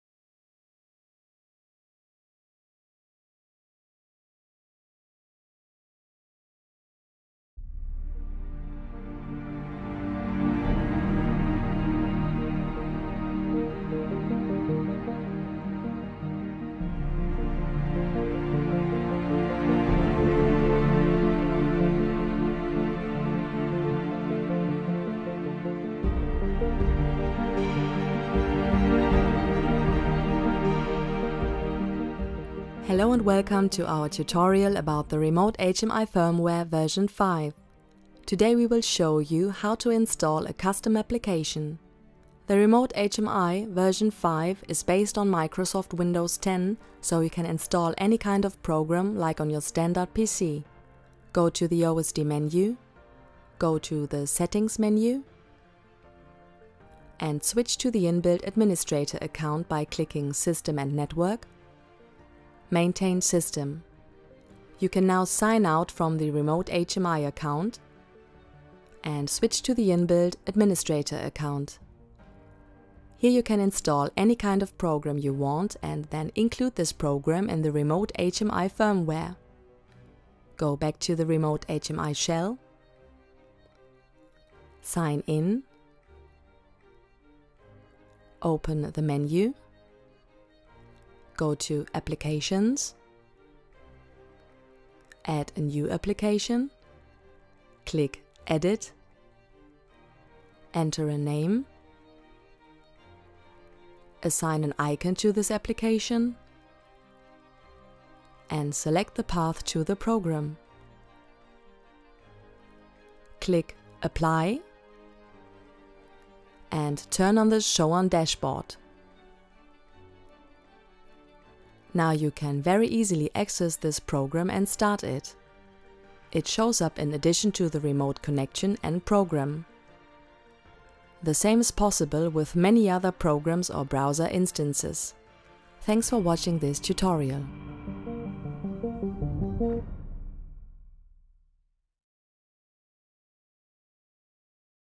REMOTE HMI V5 - Setting up specific applications
The option of setting up specific, customized applications is one of many provided by R.STAHL's Windows-10-based, app-controlled Remote HMI V5 firmware for Thin Clients. This tutorial shows you how it's done.

The track was changed in volume and shortened in the duration.